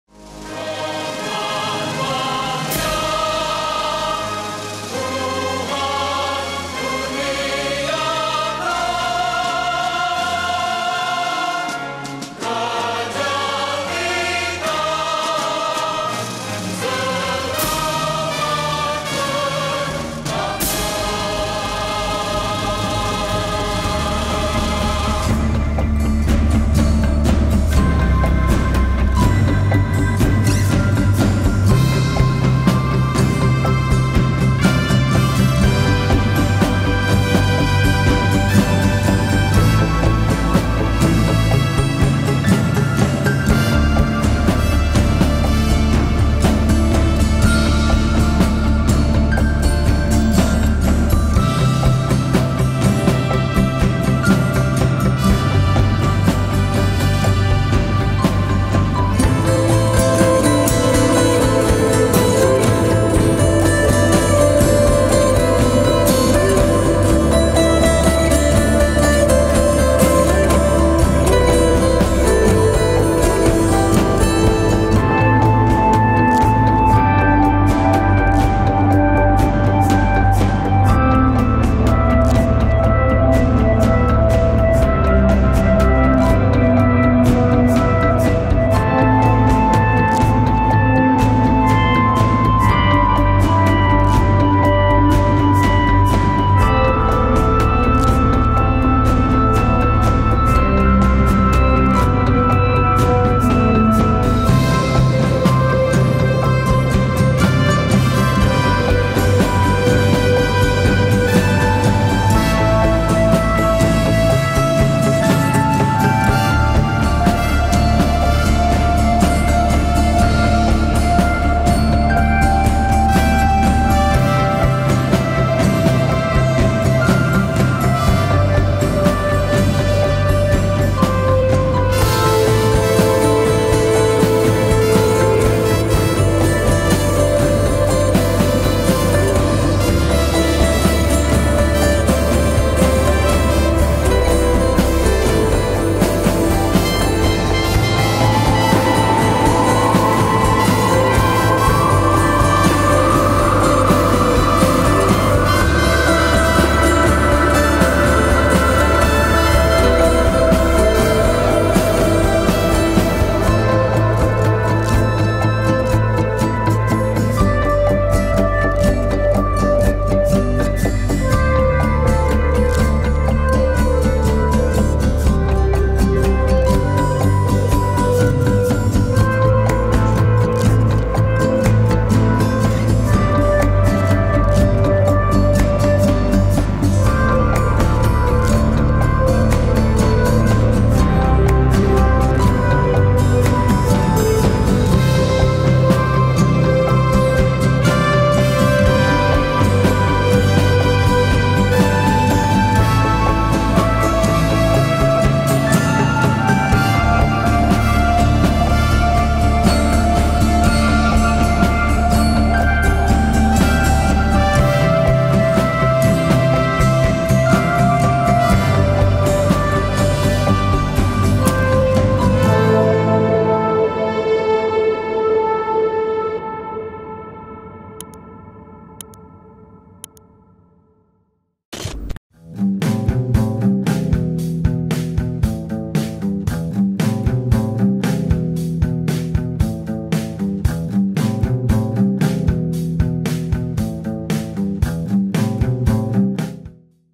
20 Janji Tanah Melayu Kepada Sabah Sewaktu Pembentukan Malaysia
“Dua Puluh Perkara” merupakan memorandum 20 perkara yang dikemukan oleh Sabah ketika Perjanjian Malaysia 1963 MA63 untuk menjamin Hak dan kepentingan Rakyat Sabah. Ini intipatinya yang perlu diingati.